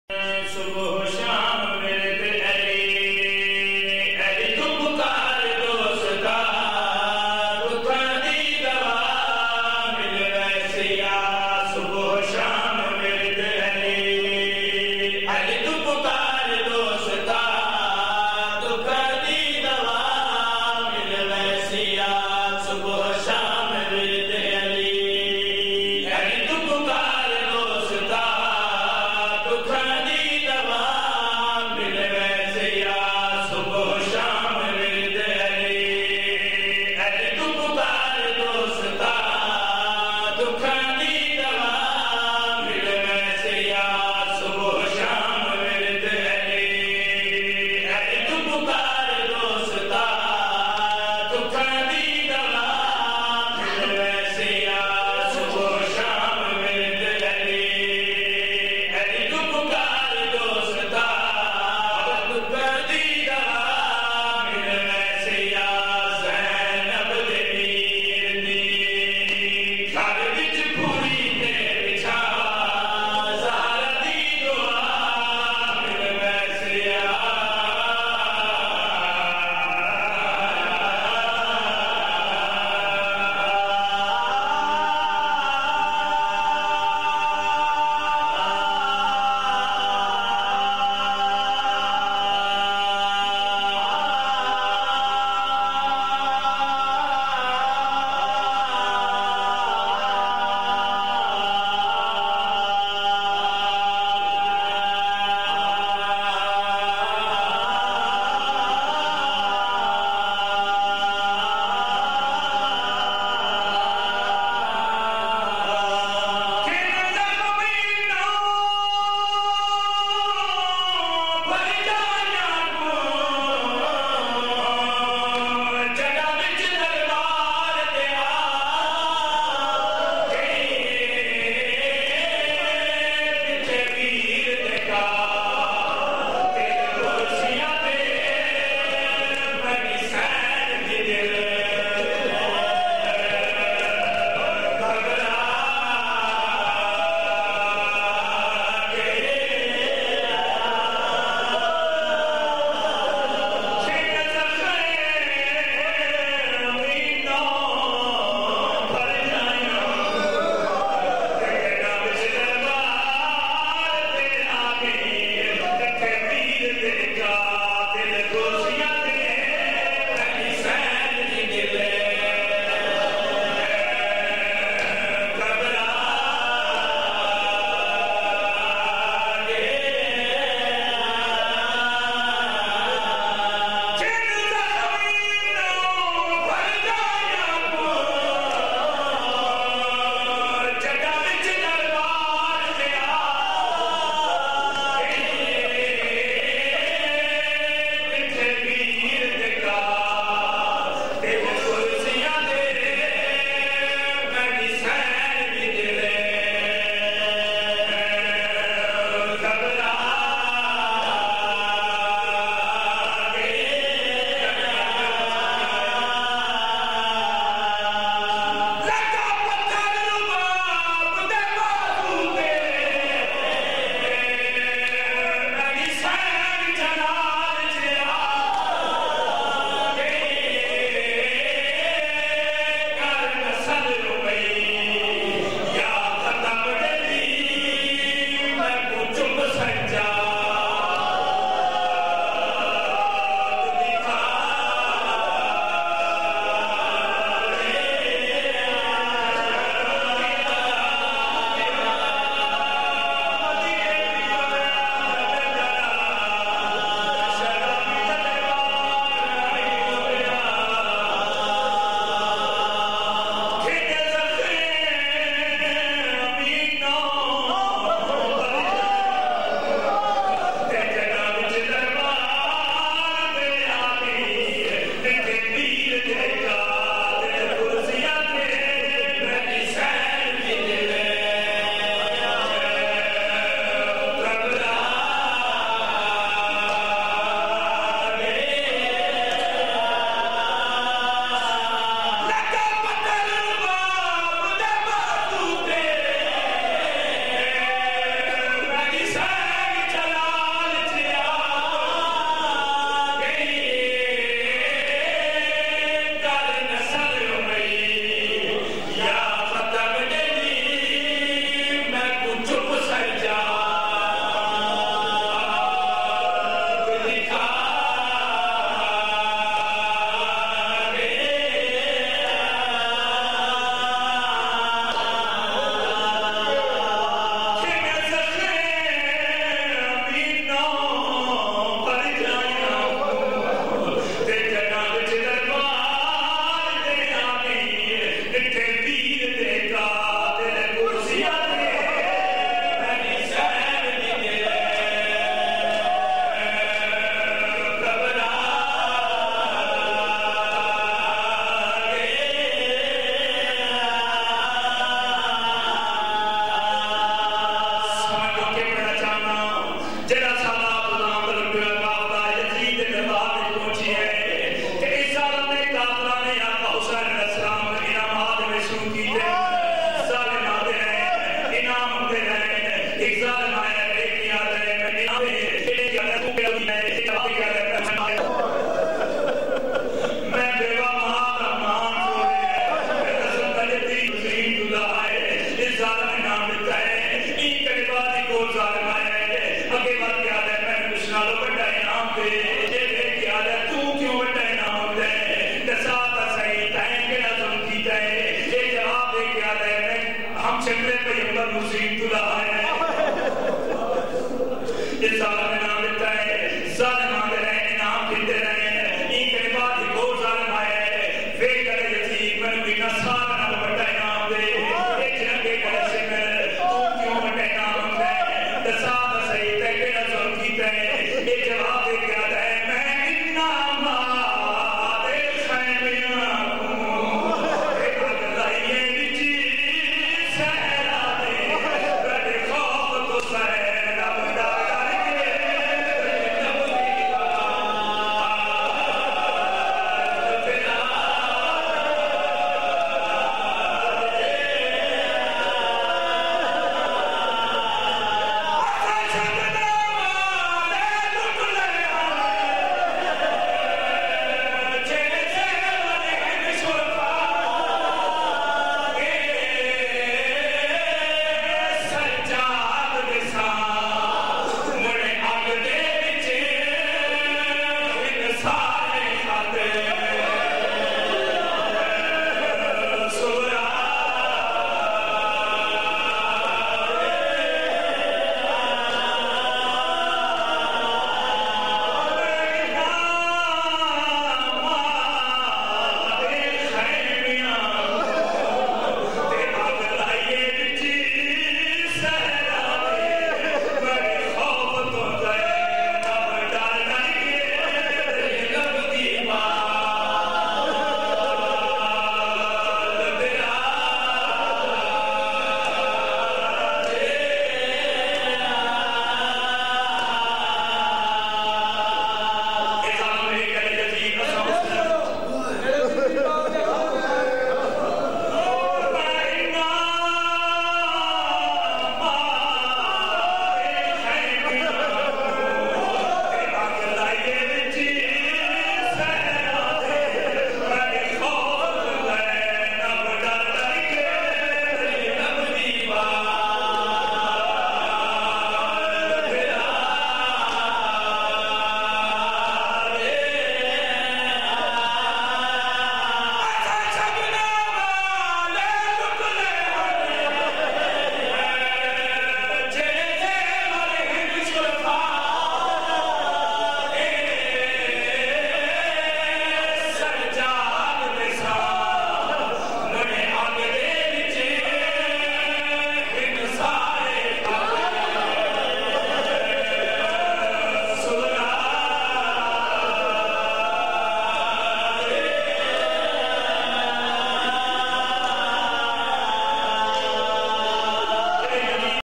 Zakir Ali Ghafar Kaosar
13 safar 2003    Markzi imambargah Gujar Khan                     Nazim-e-Majlis (bawa Murrawat Hussain Shah Bukhri)   (bawa Farman Ali Shah saib)(bawa Syed Akhtar Imam Rizvi Saib)